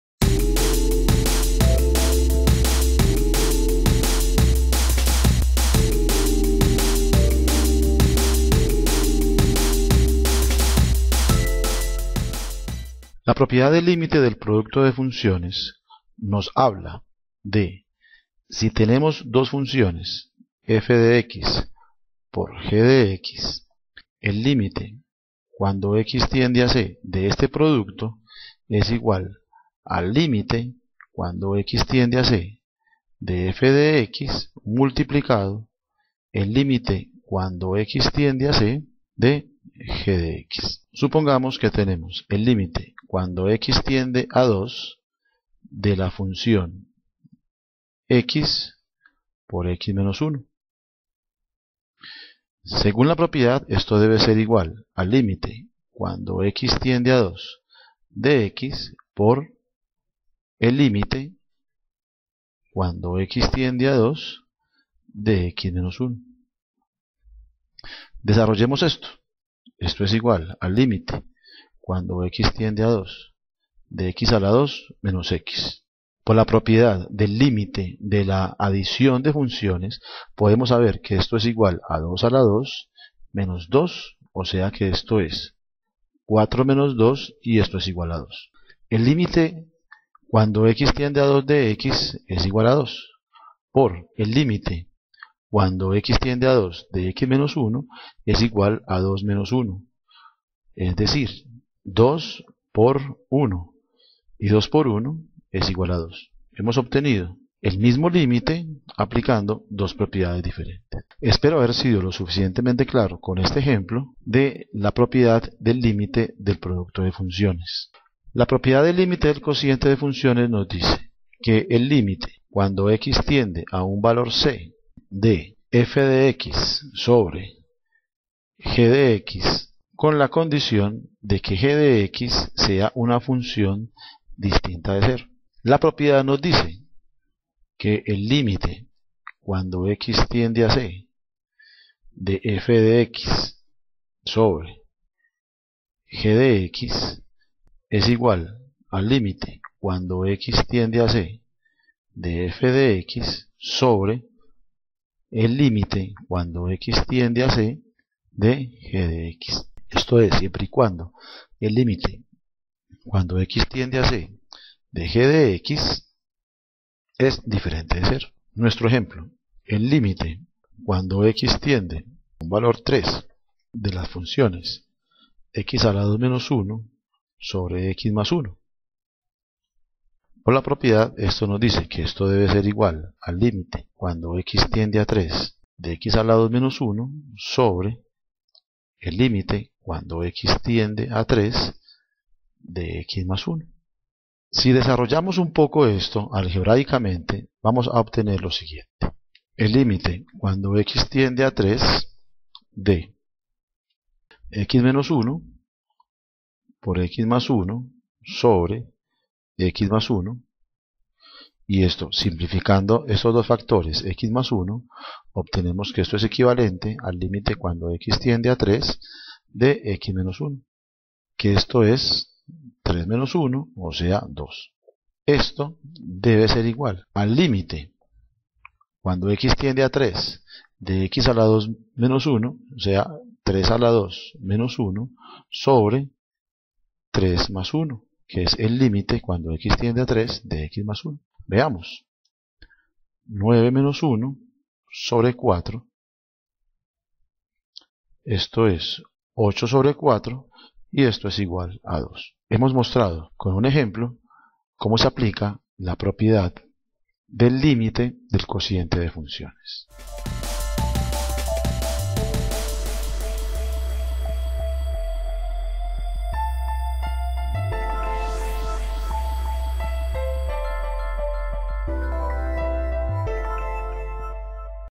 Propiedades de los límites producto y cociente)
Se inicia con la definición de un producto de funciones y se halla el límite de la función producto, realizando el producto y aplicando propiedades de la adición de funciones, paralelamente, con las propiedades del producto de los límites, para llegar al mismo resultado.  De forma análoga, se determina el límite del cociente de funciones, donde se realiza inicialmente, la simplificación de la función racional, paralelamente a la aplicación de la propiedad del cociente de los límites, y verificar que se obtiene el mismo resultado. TECNOLOGIA
INNOVACION
CULTURA
VIDA

2013